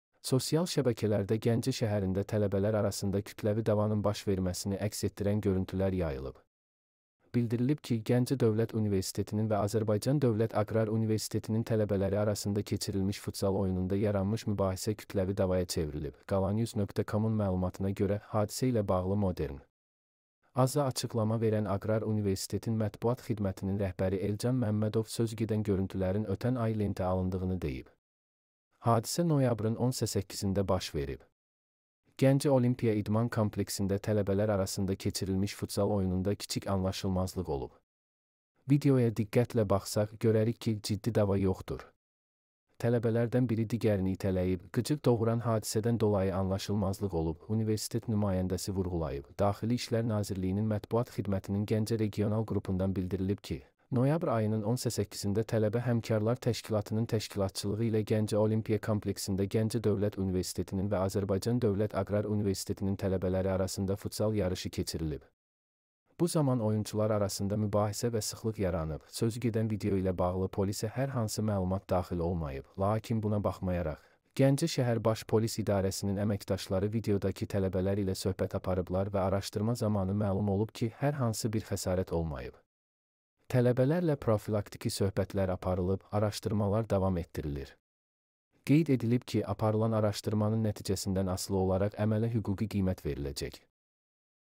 Universitetdən kütləvi dava
Sosial şəbəkələrdə Gəncə şəhərində tələbələr arasında kütləvi davanın baş verməsini əks etdirən görüntülər yayılıb. Bildirilib ki, Gəncə Dövlət Universitetinin və Azərbaycan Dövlət Aqrar Universitetinin tələbələri arasında keçirilmiş futzal oyununda yaranmış mübahisə kütləvi davaya çevrilib.

QalaNews.com -un məlumatına görə, hadisə ilə bağlı açıqlama verən Aqrar Universitetin mətbuat xidmətinin rəhbəri Elcan Məmmədov sözügedən görüntülərin ötən ay lentə alındığını deyib.

“Hadisə noyabrın 18-də baş verib. Gəncə Olimpiya İdman Kompleksində tələbələr arasında keçirilmiş futzal oyununda kiçik anlaşılmazlıq olub. Videoya diqqətlə baxsaq, görərik ki, ciddi dava yoxdur. Tələbələrdən biri digərini itələyib, qıcıq doğuran hadisədən dolayı anlaşılmazlıq olub”, - universitet nümayəndəsi vurğulayıb.

DİN-in Mətbuat Xidmətinin Gəncə regional qrupundan bildirilib ki, noyabr ayının 18-da Tələbə Həmkarlar Təşkilatının təşkilatçılığı ilə Gəncə Olimpiya Kompleksində Gəncə Dövlət Universitetinin və Azərbaycan Dövlət Aqrar Universitetinin tələbələri arasında futzal yarışı keçirilib. Bu zaman oyunçular arasında mübahisə və sıxlıq yaranıb:

“Sözügedən video ilə bağlı polisə hər hansı məlumat daxil olmayıb, lakin buna baxmayaraq, Gəncə Şəhər Baş Polis İdarəsinin əməkdaşları videodakı tələbələr ilə söhbət aparıblar və araşdırma zamanı məlum olub ki, hər hansı bir xəsarət olmayıb. Tələbələrlə profilaktiki söhbətlər aparılıb, araşdırmalar davam etdirilir”.

Qeyd edilib ki, aparılan araşdırmanın nəticəsindən asılı olaraq əmələ hüquqi qiymət veriləcək.